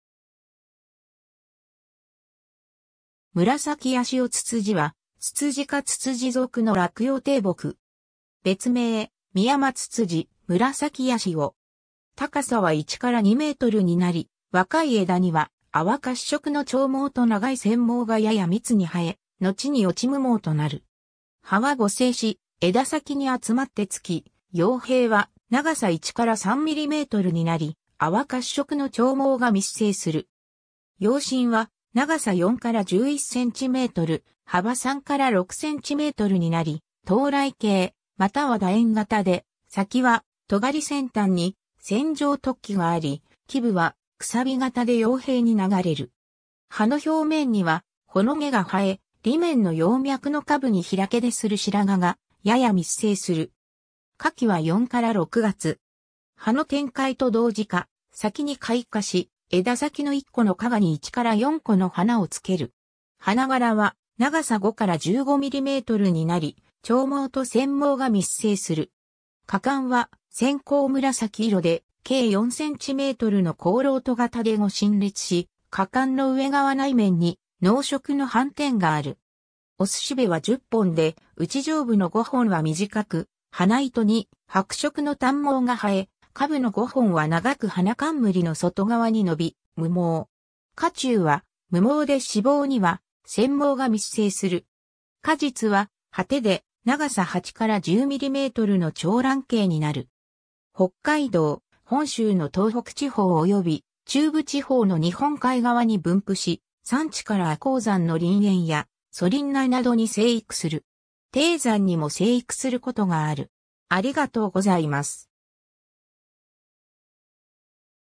ムラサキヤシオツツジ
ムラサキヤシオツツジ（紫八汐躑躅、学名：Rhododendron albrechtii）はツツジ科ツツジ属の落葉低木。
別名、ミヤマツツジ、ムラサキヤシオ。
 高さは1-2mになり、若い枝には淡褐色の長毛と長い腺毛がやや密に生え、のちに落ち無毛となる。
 葉は互生し、枝先に集まってつき、葉柄は長さ1-3mmになり、淡褐色の長毛が密生する。
葉身は長さ4-11cm、幅3-6cmになり、倒卵形または楕円形で、先はとがり先端に腺状突起があり、基部はくさび形で葉柄に流れる。
葉の表面には微毛が生え、裏面の葉脈の下部に開出する白毛がやや密生する。
葉の展開と同時か先に開花し、枝先の1個の花芽に1-4個の花をつける。
花柄は長さ5-15mmになり、長毛と腺毛が密生する。
花冠は鮮紅紫色で、径4cmの広漏斗形で5深裂し、花冠の上側内面に濃色の斑点がある。
雄蘂は10本で、うち上部の5本は短く、花糸に白色の短毛が生え、下部の5本は長く花冠の外側に伸び、無毛。
花柱は無毛で子房には腺毛が密生する。
果実は蒴果で長さ8-10mmの長卵形になる。
 北海道、本州の東北地方および中部地方の日本海側に分布し、山地から亜高山の林縁や疎林内などに生育する。
低山にも生育することがある。